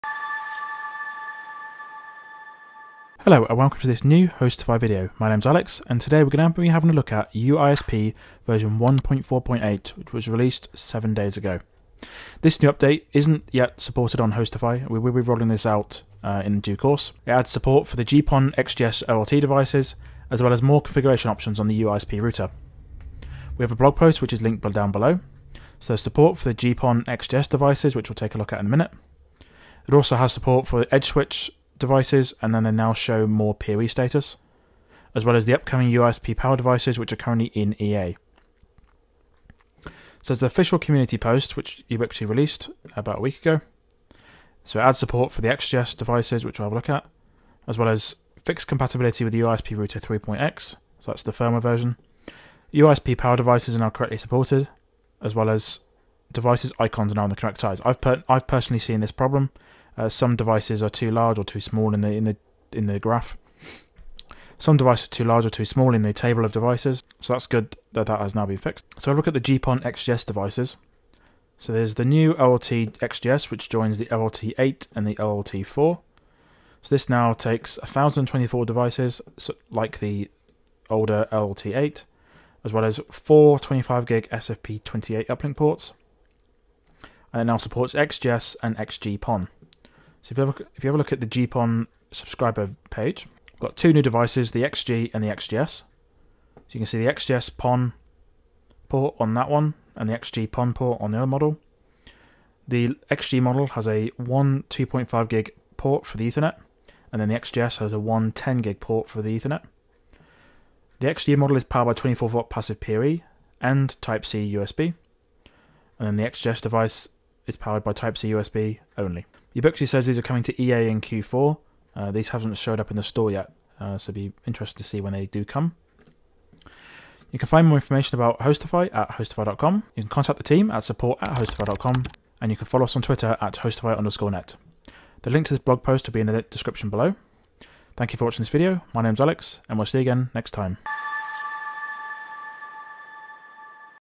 Ubiquiti releases UISP 1.4.8!
Ubiquiti has released a new update for the UISP application, bringing some usability improvements for the UISP Router, support for new UFiber OLT devices and many bug fixes.

Connect with HostiFi